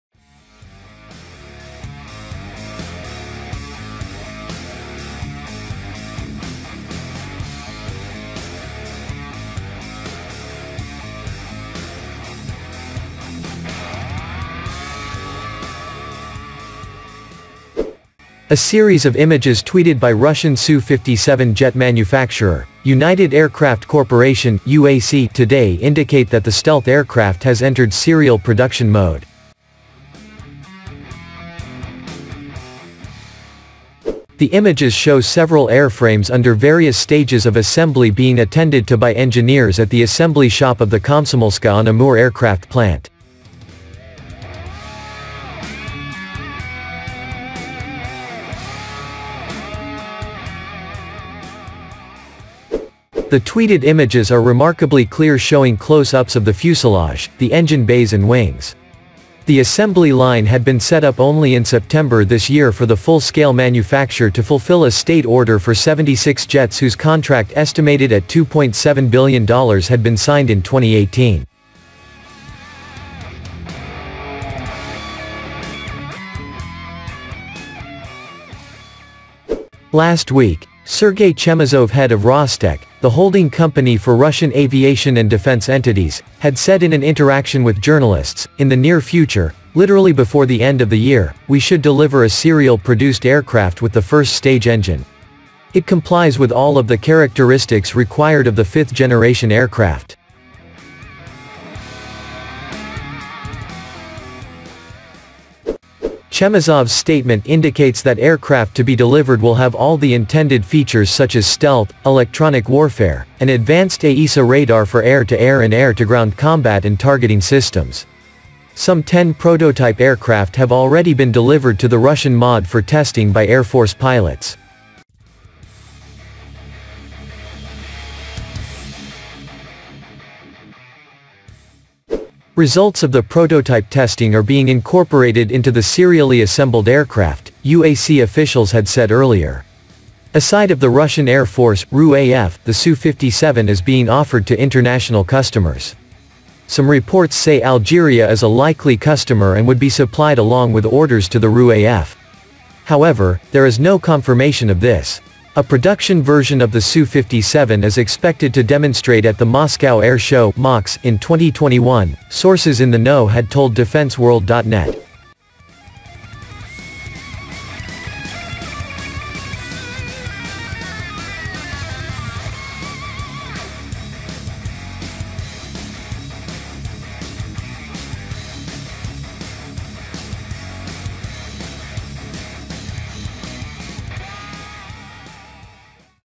Russian Su-57 stealth aircraft have entered Russian serial production mode
A series of images tweeted by Russian Su-57 jet manufacturer, United Aircraft Corporation (UAC) today indicate that the stealth aircraft has entered serial production mode.

The images show several airframes under various stages of assembly being attended to by engineers at the assembly shop of the Komsomolsk-on-Amur Aircraft Plant. The tweeted images are remarkably clear showing close-ups of the fuselage, the engine bays and wings.

The assembly line had been set up only in September this year for the full-scale manufacture to fulfill a state order for 76 jets whose contract estimated at $2.7 billion had been signed in 2018.